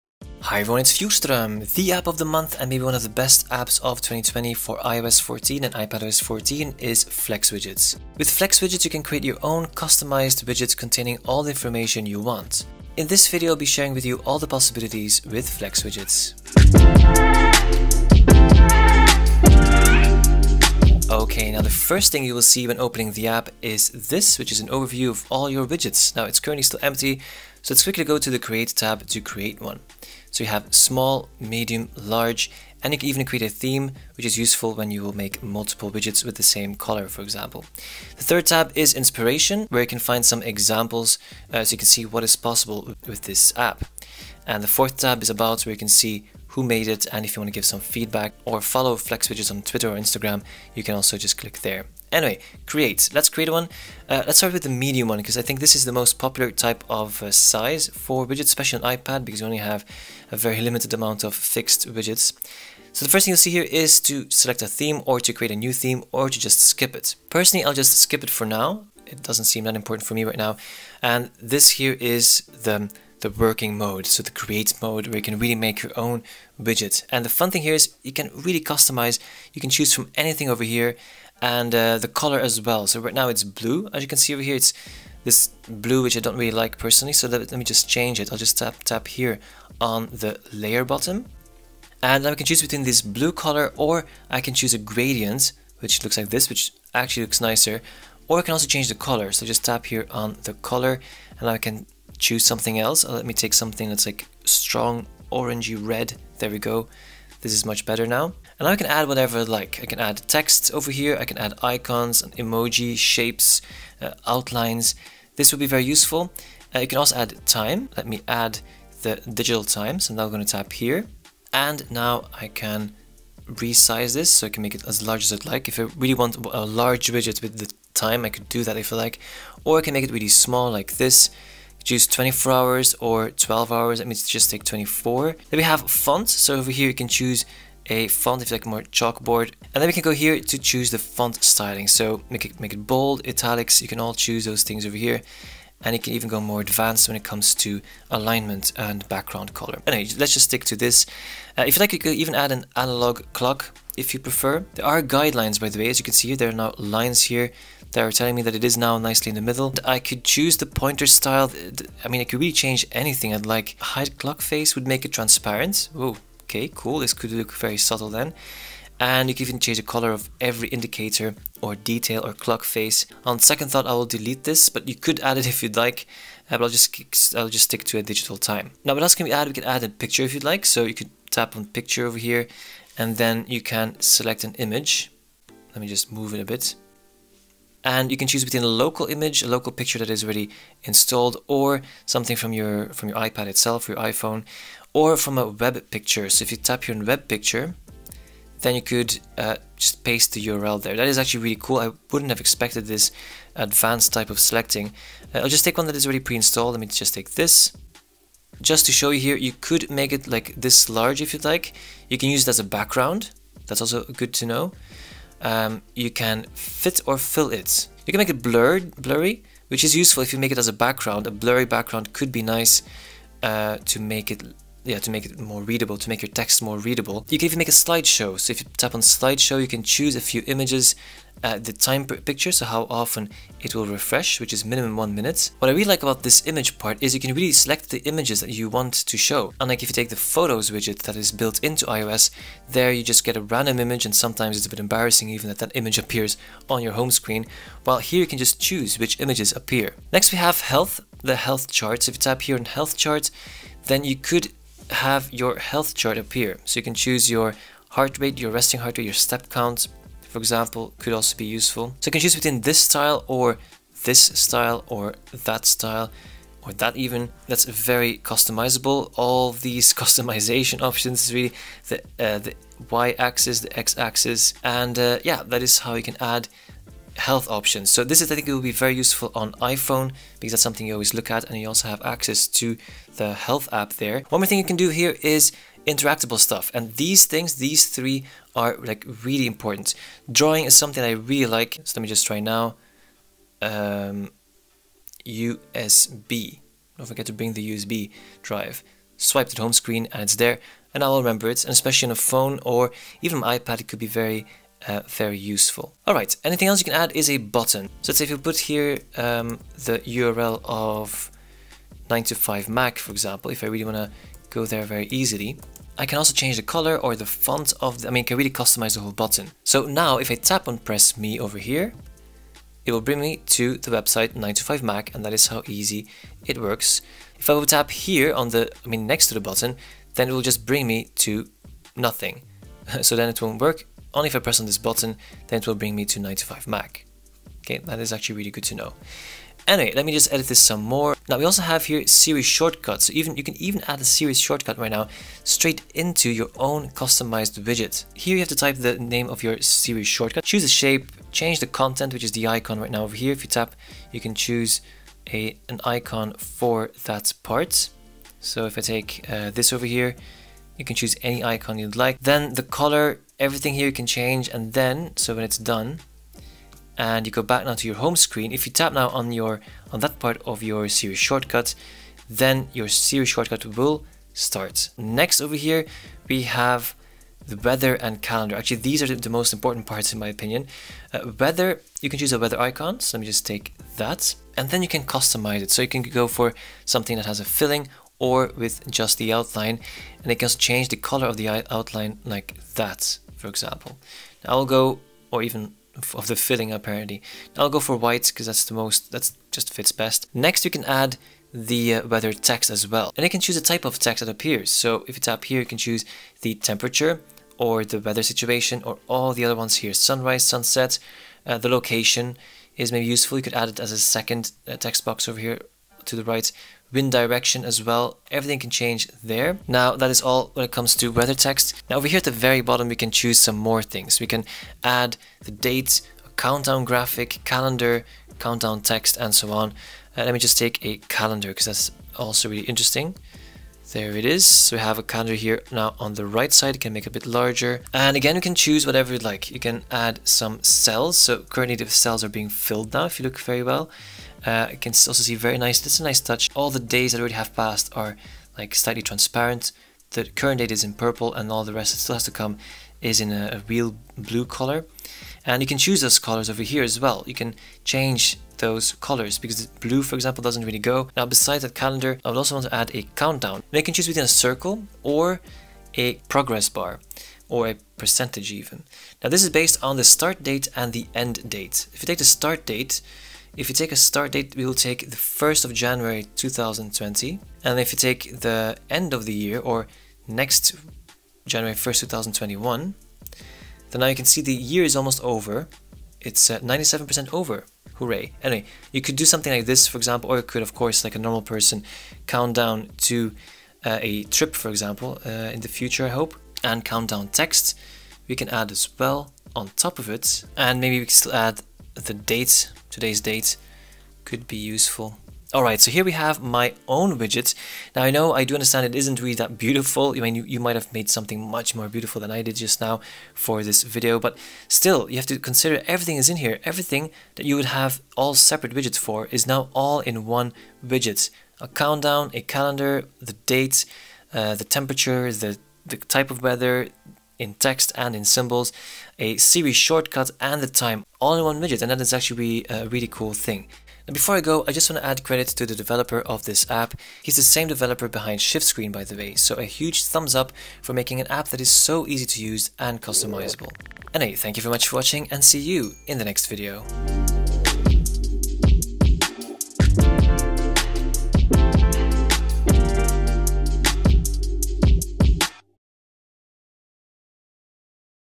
NEW widget app for iPad and iPhone! | MAKE YOUR OWN FLEX WIDGETS | DECEMBER 2020
Flex Widgets, the best app to create your own customised widgets on iOS 14 and iPadOS 14.
Music licensed for this video with Soundstripe.